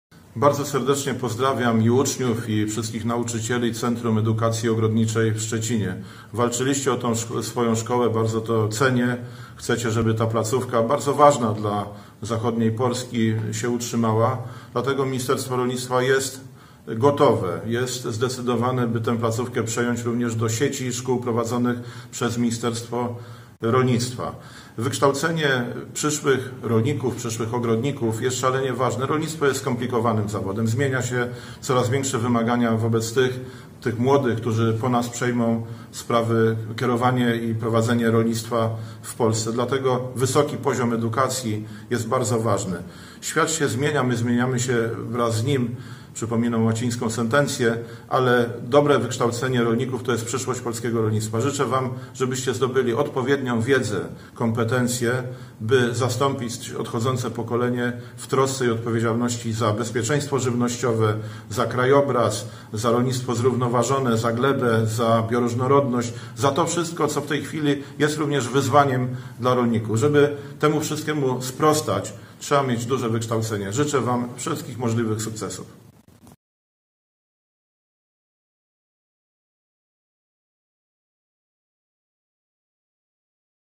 CEO Szczecin - wystąpienie Ministra Rolnictwa i Rozwoju Wsi [29.08.2019 r.]
Wystąpienie p. Jana Krzysztofa Ardanowskiego, Ministra Rolnictwa i Rozwoju Wsi adresowane do Społeczności Szkolnej Centrum Edukacji Ogrodniczej w Szczecinie. Realizacja: Warszawa, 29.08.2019 r. Oprac. ZRW NSZZ Rolników Indywidualnych "Solidarność"